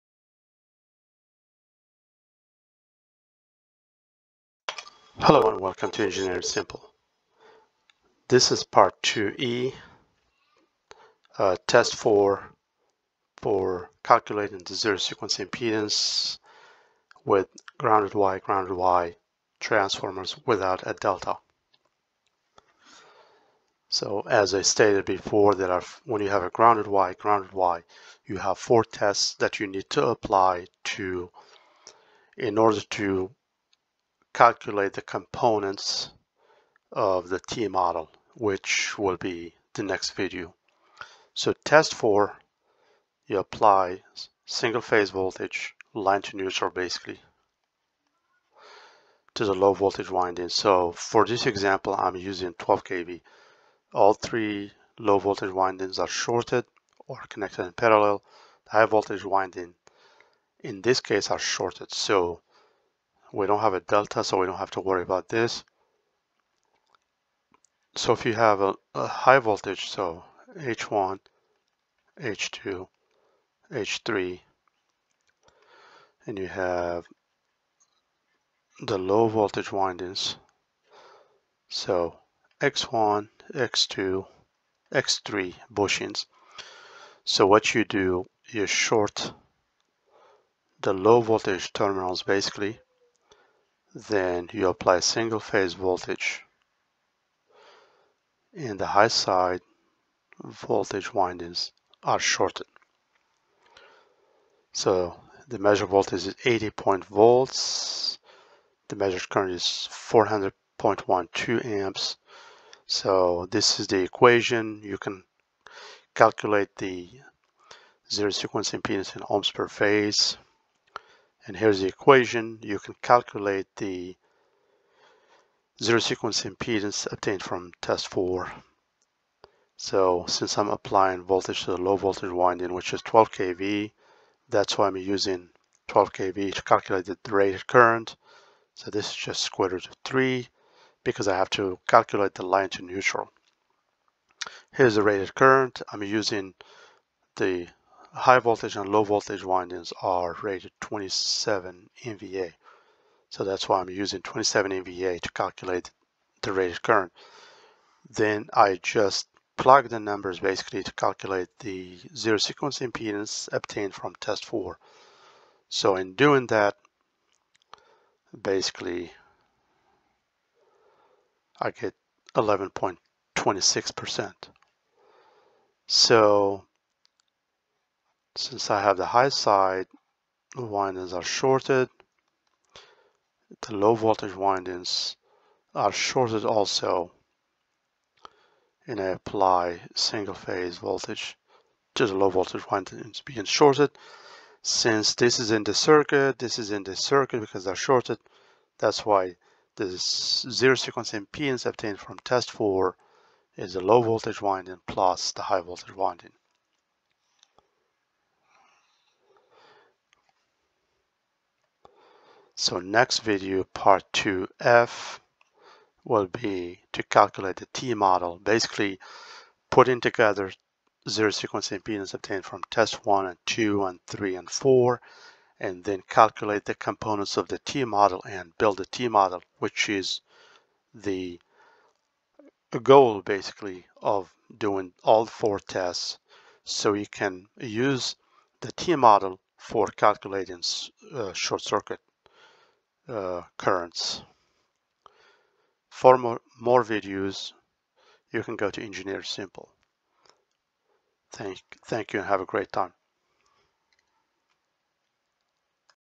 Transformer Zero Sequence Impedance Testing YNyn Part 02 E Test#4
Welcome to Engineering Simple!

Test #4 is the last test performed on grounded wye-grounded wye transformers to calculate zero sequence impedances required to build the equivalent T-model for 3 phase, 3 leg core transformers. This video provides an example of the calculation and connection for the test.